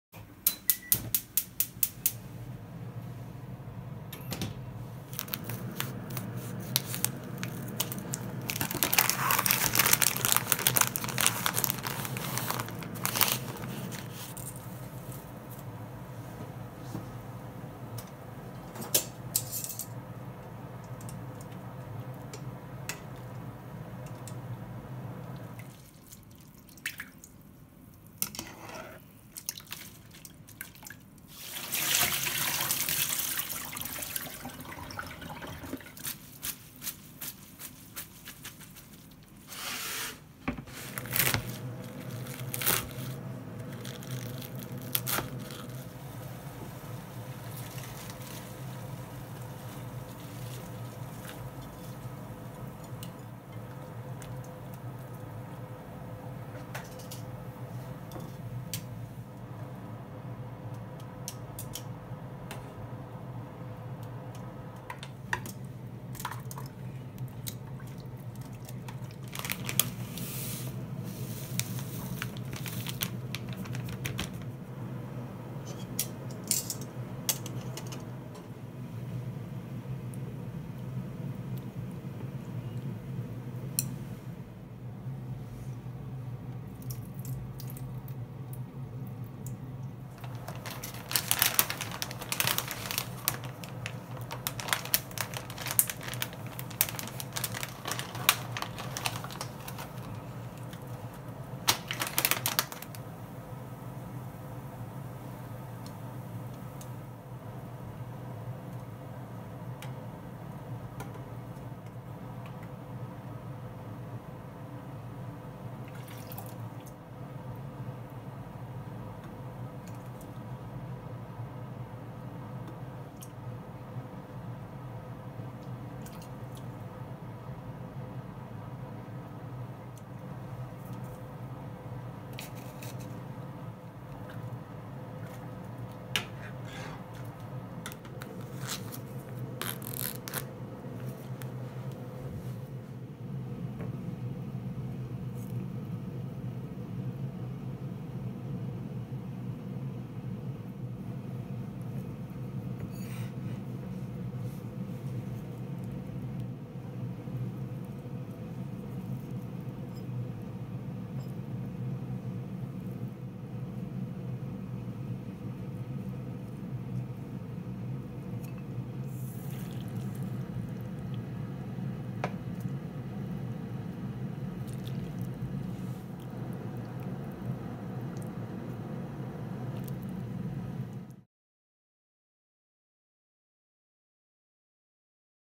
#03 Housewife cooking routine - Yuba Soba
I’m Thai and live in Japan
As a housewife, I have to cook everyday. I always cook both Japanese and Thai food (or sometimes Italian, Korean or Chinese food too)

This’s what I cooked

Dinner
- Yuba Soba